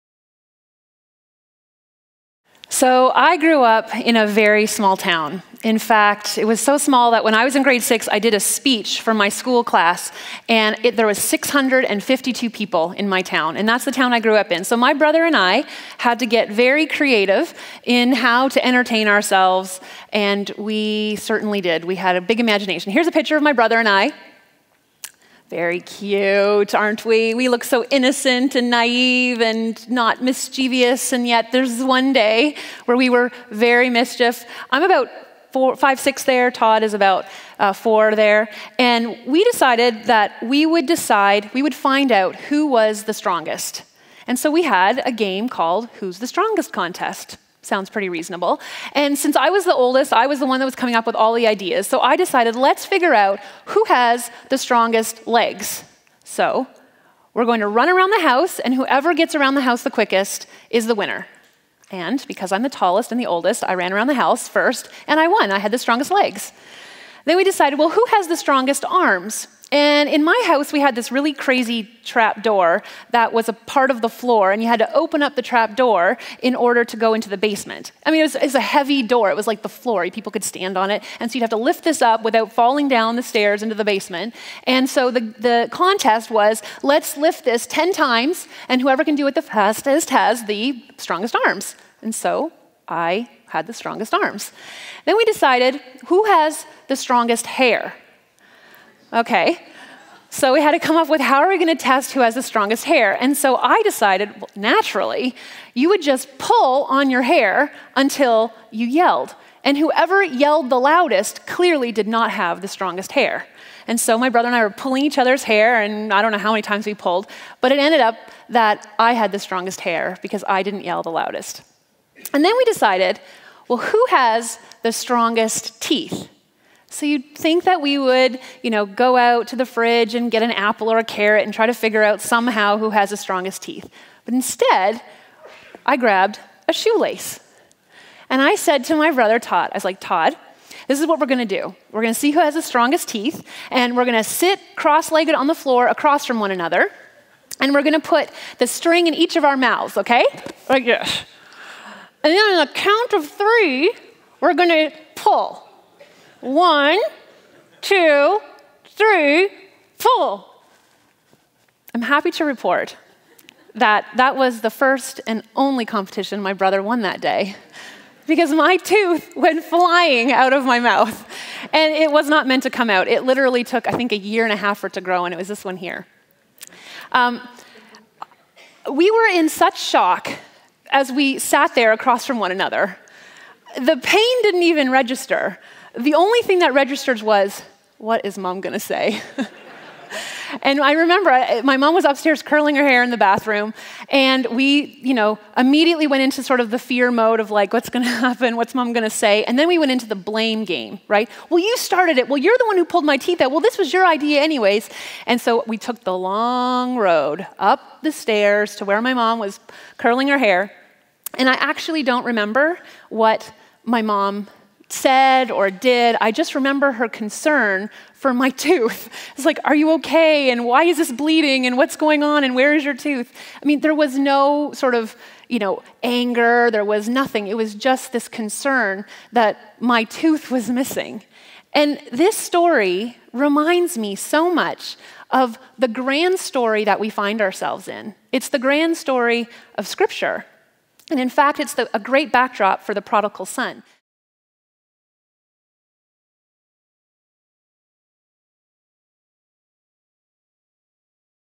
Who's The Strongest?
Here are some highlights from Christa's Message "The Young Son's Journey" - the second message in our Coming Home series.